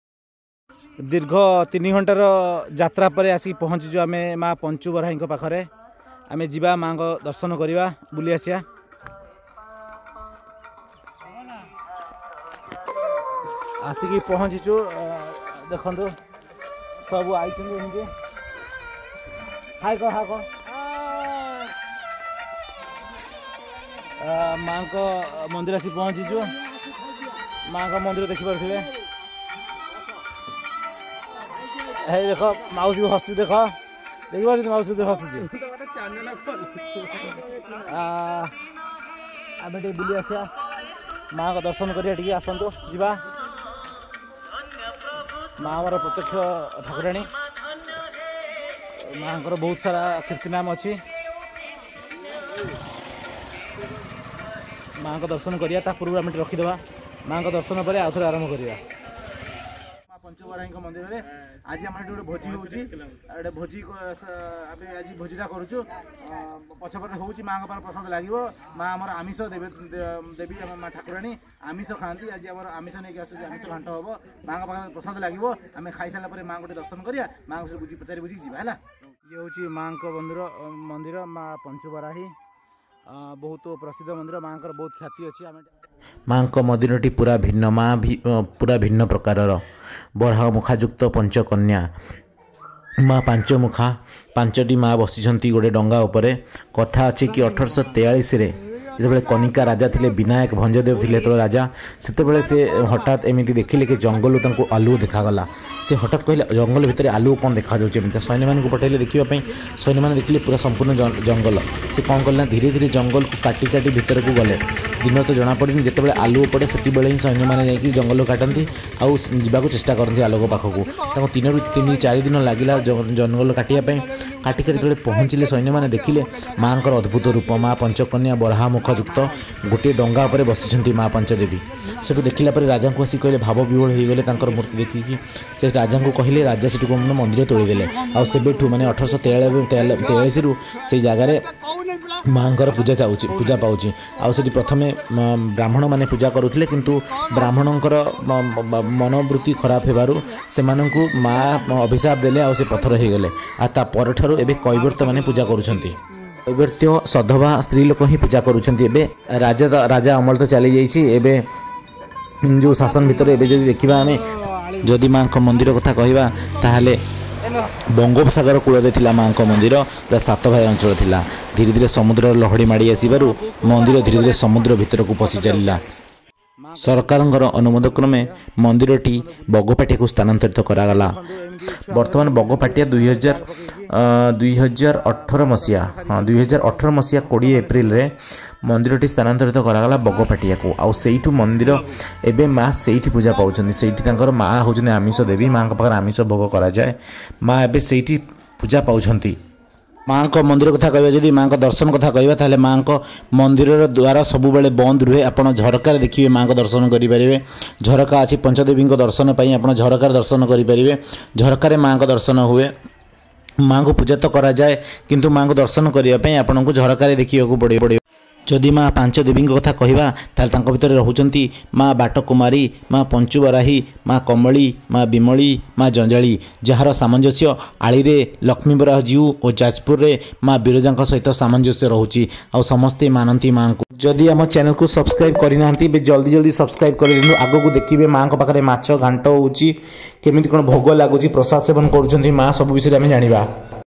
Maa Panchubarahi Temple Satabhaya, Rajnagar | God power | ସାତଭାୟାରୁ ବଗପଟିଆର ଯାତ୍ରା
Maa Panchubarahi Temple in Satabhaya, Rajnagar Divine power | ସାତଭାୟାରୁ ବଗପଟିଆର ଯାତ୍ରା

ମା ପଞ୍ଚୁବରାହୀ ପ୍ରତ୍ୟକ୍ଷ ଦେବୀ ଠାକୁରାଣୀ
ଅଦ୍ଭୁତ ଶକ୍ତିର ଅଧିକାରୀ
Story telling
Odisha ra puratana mandira
Odisha ra garba o gouraba
ମାଆଙ୍କର ପୂଜାର୍ଚ୍ଚନା ଆଉ ଅଲୌକିକ ଶକ୍ତି ଦେଖିବାକୁ ଲୋକଙ୍କ ଭିଡ ଯମେ

jagatjanani ମା